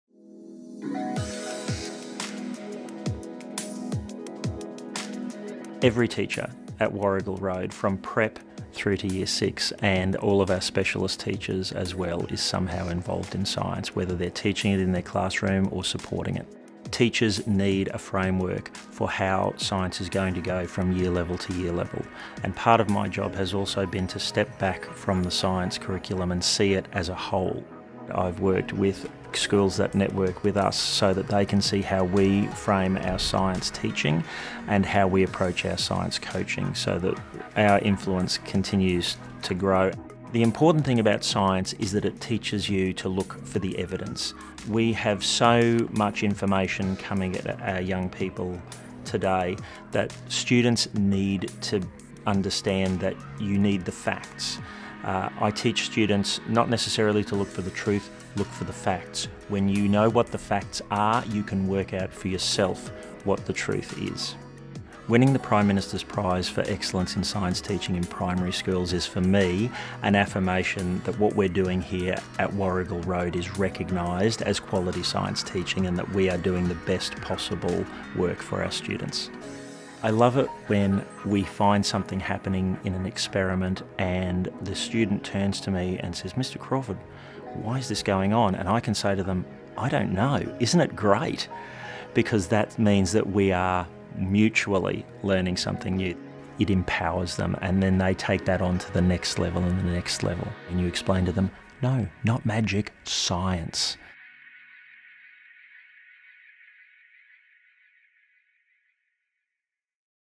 PM Prize for Excellence in Science Teaching in Primary Schools 2018 - Brett Crawford
Brett Crawford was awarded the Prime Minister’s Prize for Excellence in Science Teaching in Primary Schools in 2018 for creating an environment in which every teacher is engaged in science. The video provides an overview of his achievements.